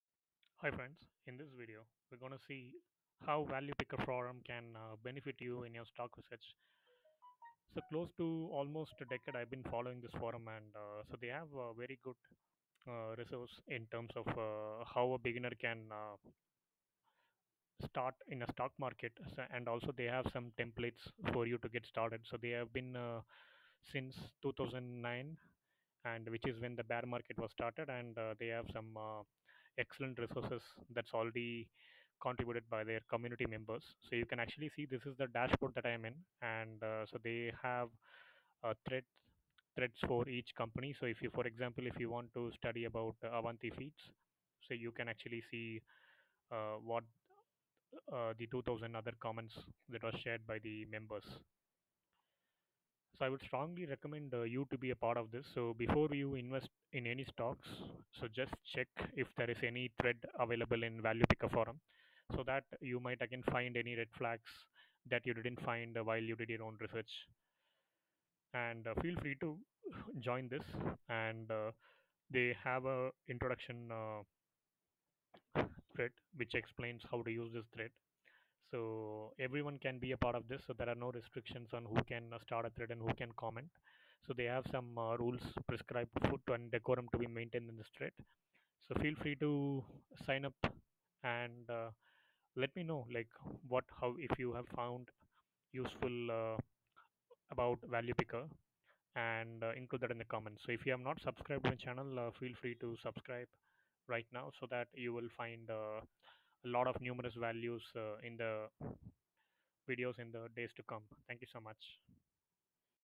ValuePickr: How to use it for Stock Research? | Saravanan Balakrishnan
ValuePickr is a forum that helps stock investors to identify opinions and research points shared by other community members.

In this video, let's find out how to use ValuePickr forum.

I hope you find this video useful and timely.